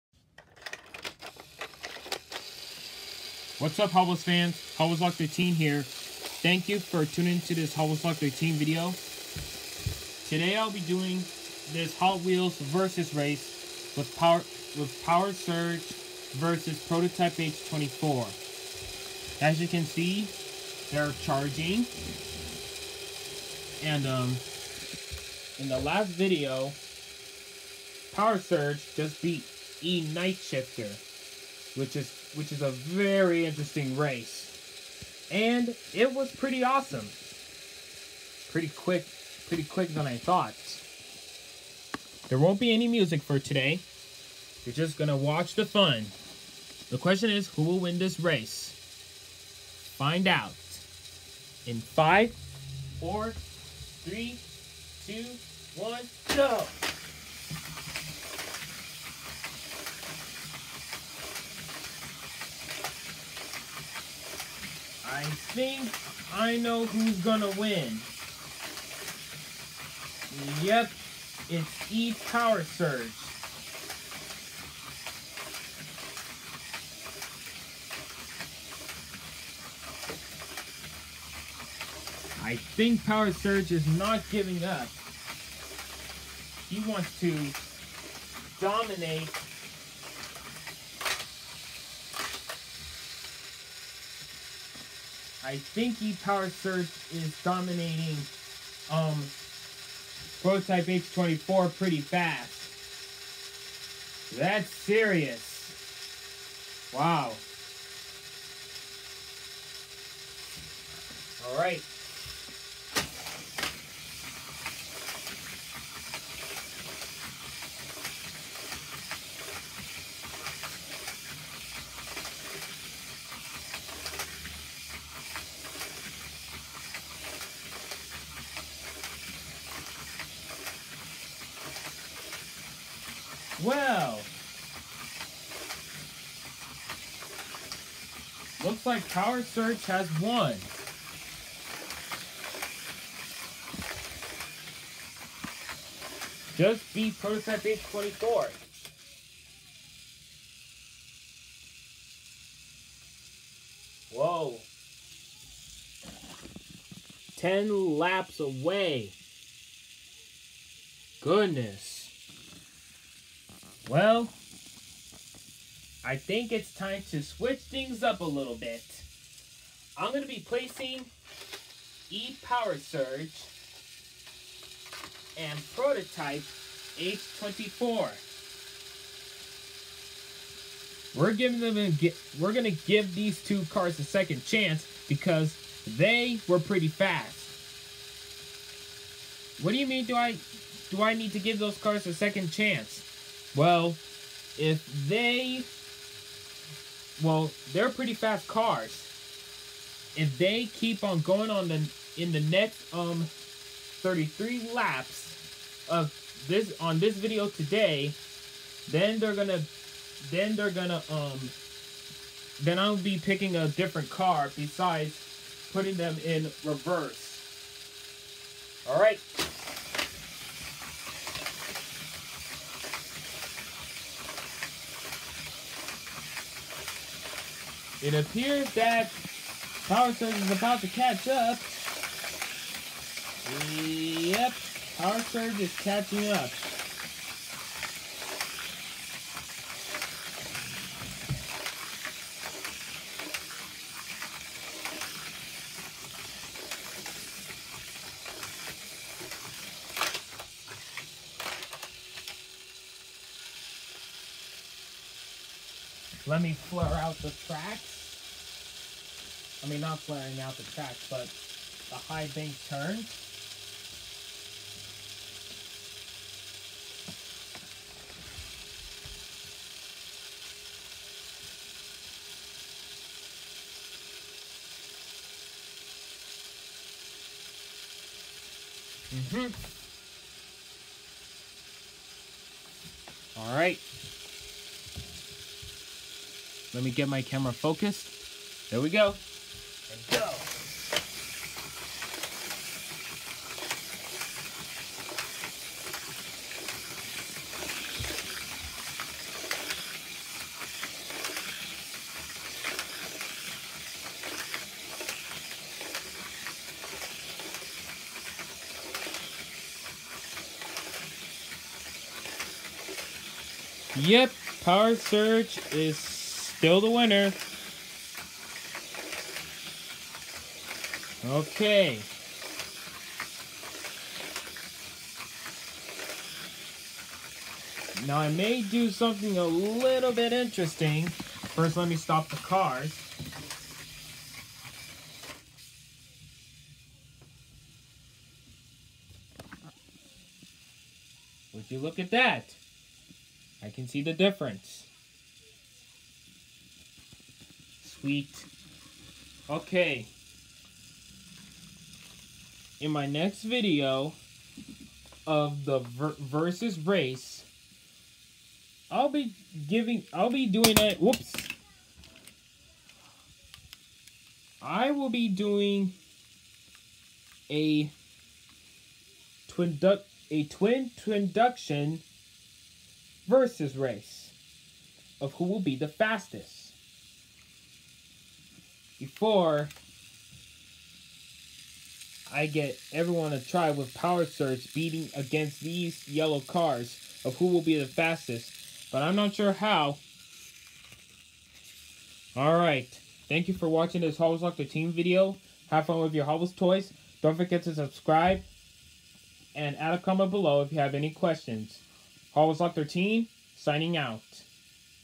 Hot Wheels Speed Chargers VS Race: PowerSurge VS Prototype H24
This video is a switching lanes video with PowerSurge and Prototype H24, because this car has been beating every racer so much, I don’t know what I want to say with PowerSurge “The Speed Demon.”